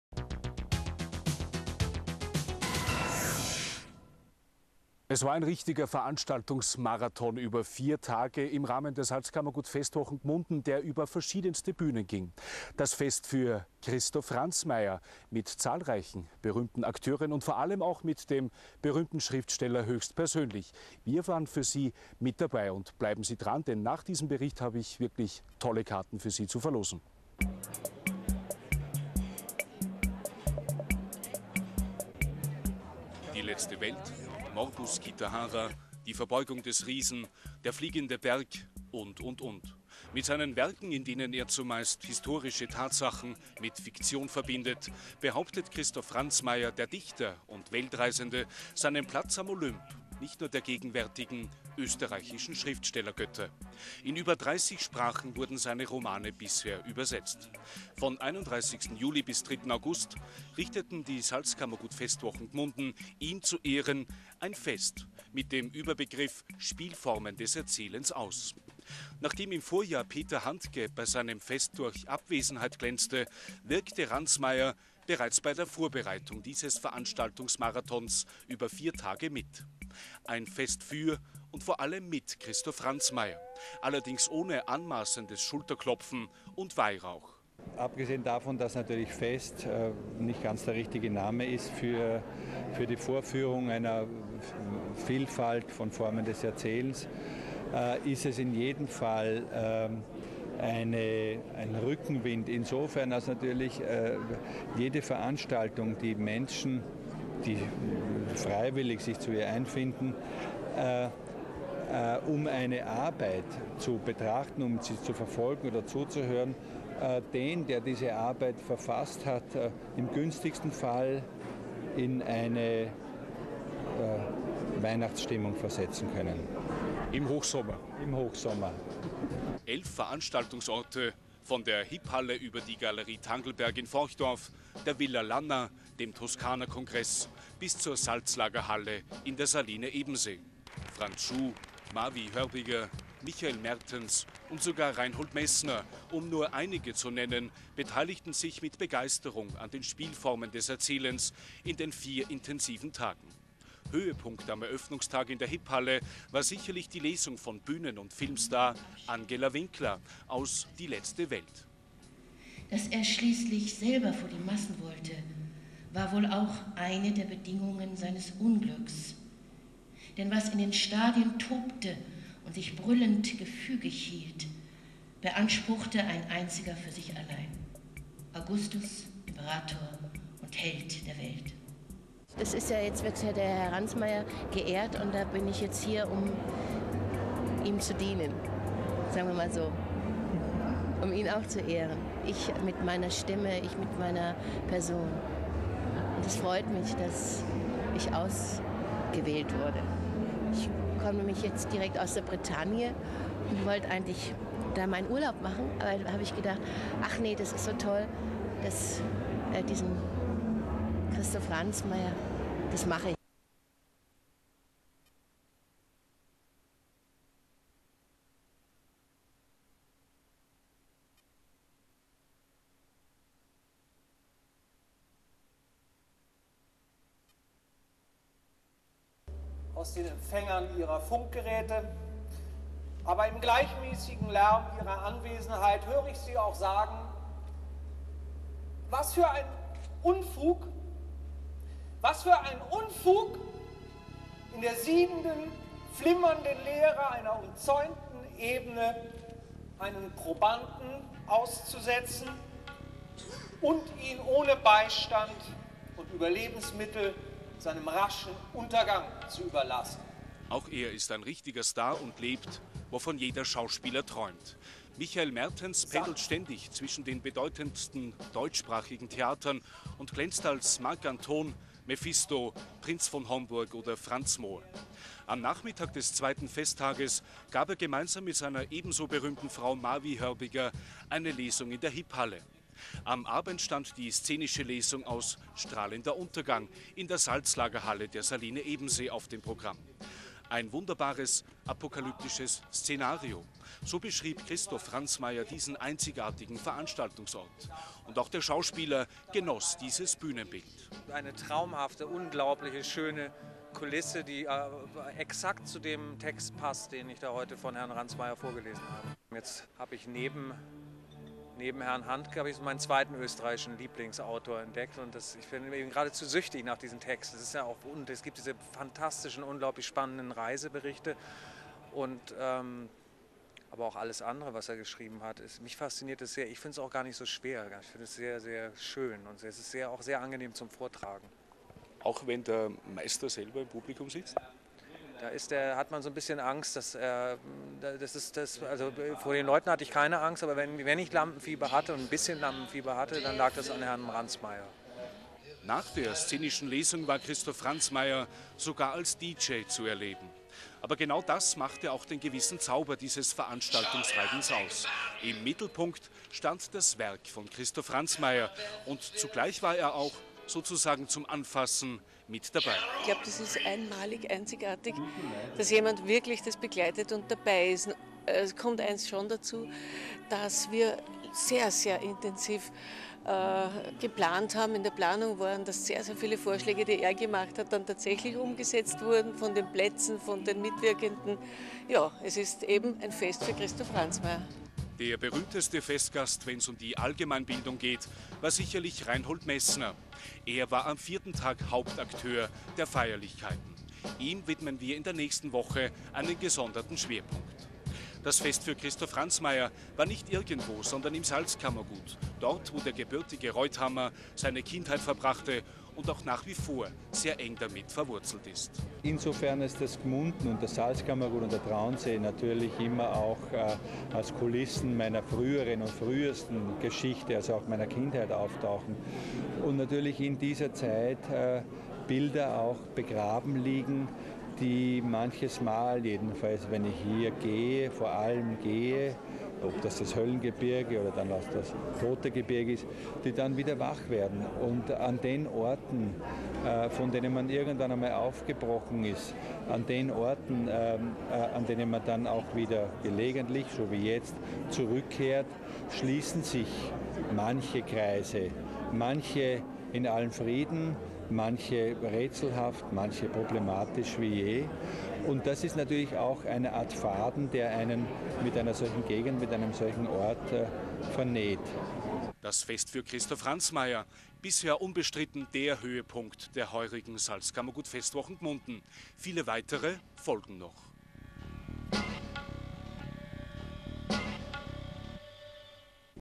Ein Fest für Christoph Ransmayr
"Ein Fest für Christoph Ransmayr" im Rahmend der Salzkammergut Festwochen Gmunden 2008, Bericht TS1